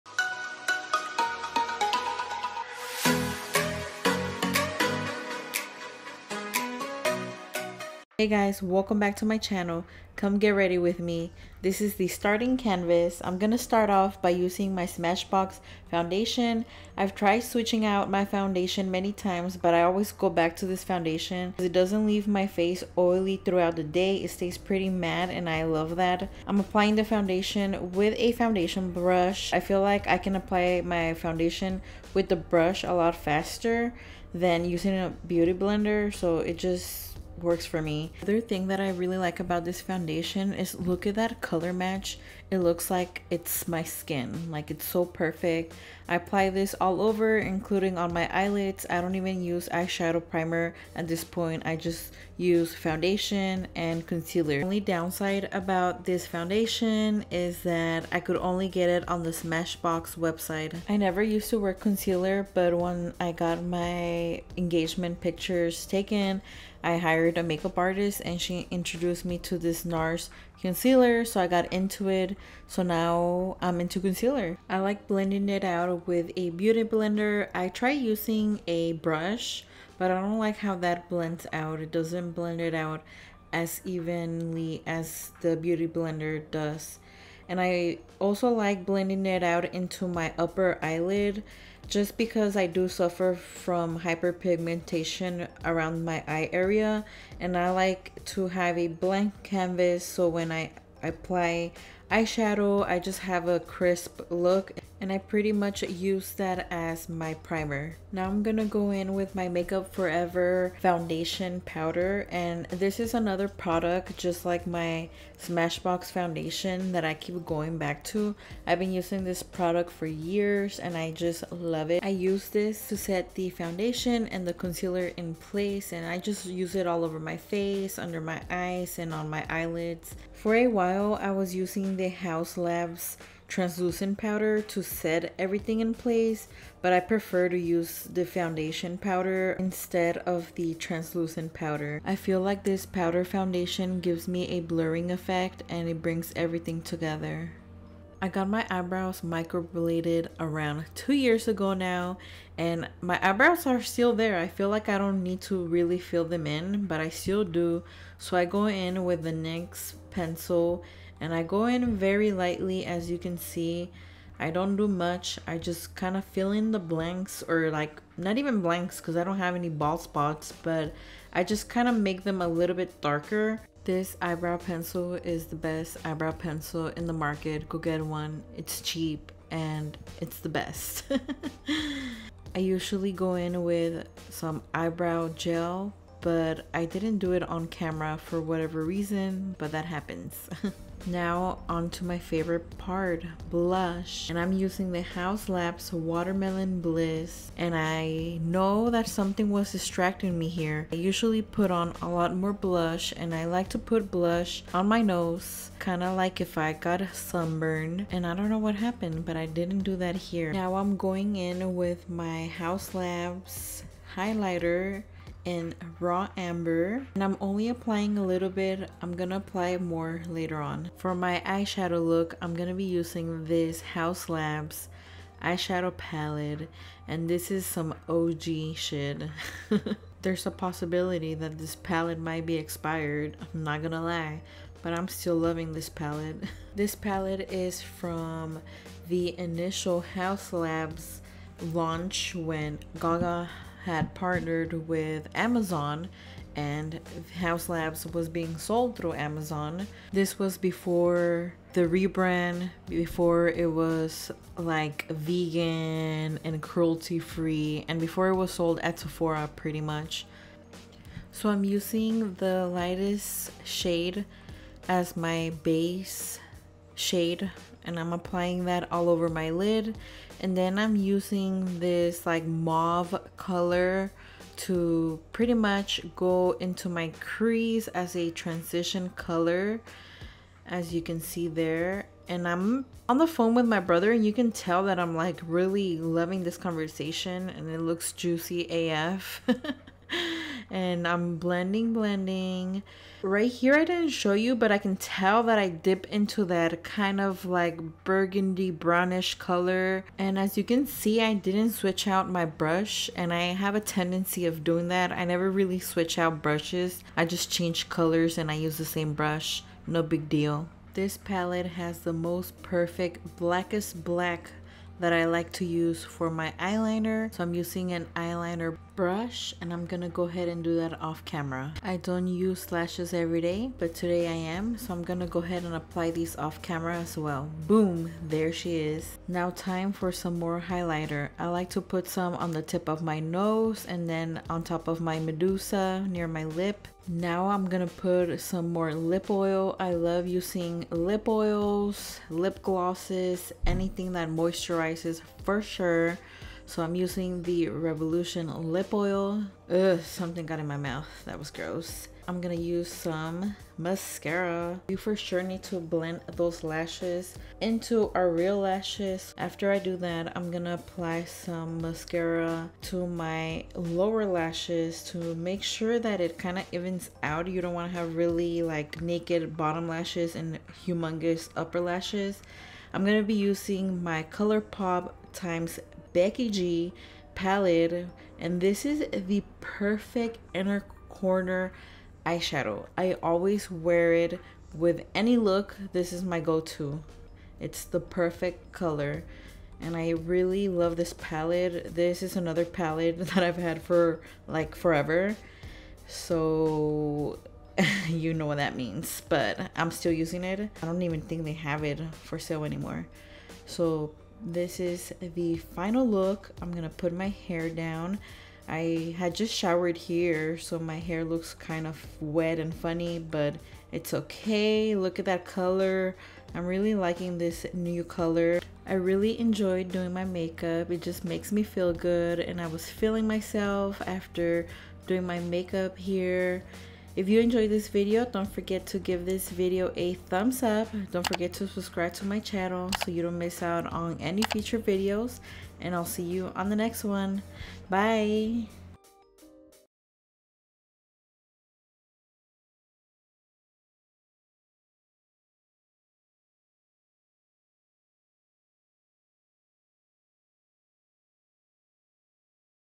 GRWM | Easy Makeup Routine
I wanted to do a GRWM using my new vanity set up.

Camaras: Nikon D5300 & Sony RX100 4